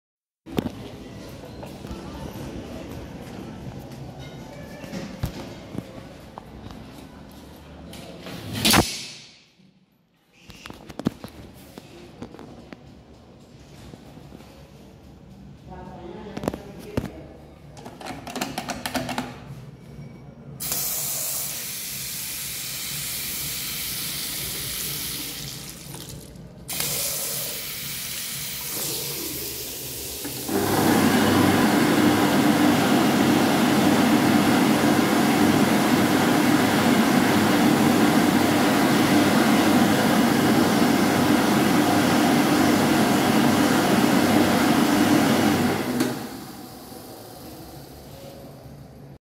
Jofel Ibero? at Yumbo Centre - Maspalomas, Gran Canaria, Canary Islands - 🚹
Here we have this Jofel Ibero I think and it turned off really quick after dryer use, and it's an average Spanish dryer btw so nothing more to say

Date recorded: May 13th 2024